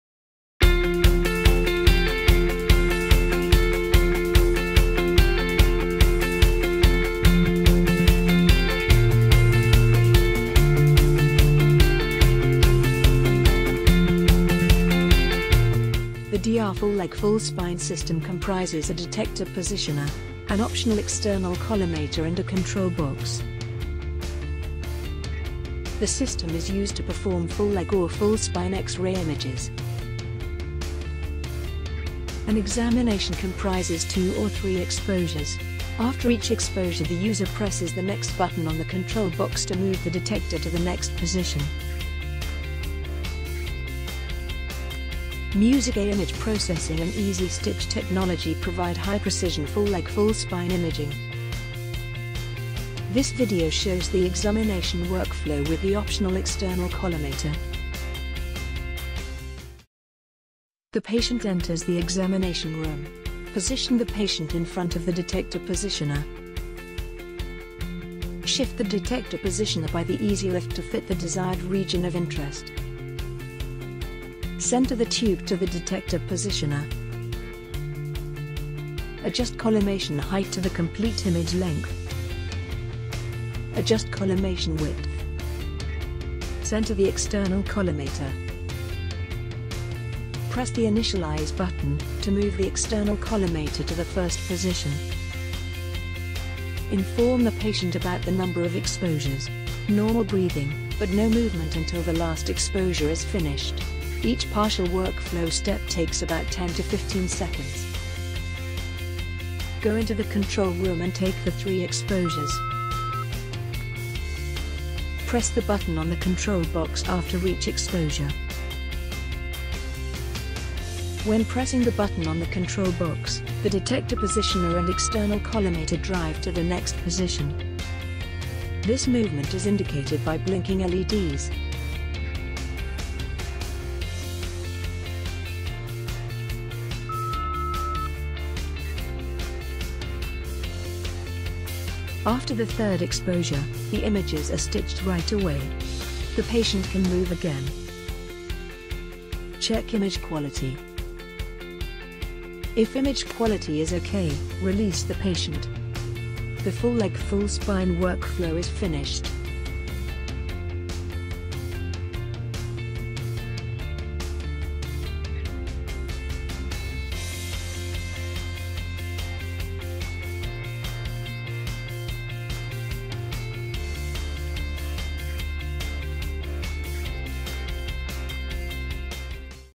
DR Retrofit FLFS Workflow with External Collimator
Lift your CR Easylift to new heights, with DR full leg/full spine imaging

Seamlessly upgrade your Agfa CR Full Leg/Full Spine (FLFS) imaging equipment with our new FLFS DR Retrofit solution. In just a moment, you can have all the speed, workflow and image quality benefits of FLFS DR imaging, while maximizing your existing equipment, including your existing Easylift!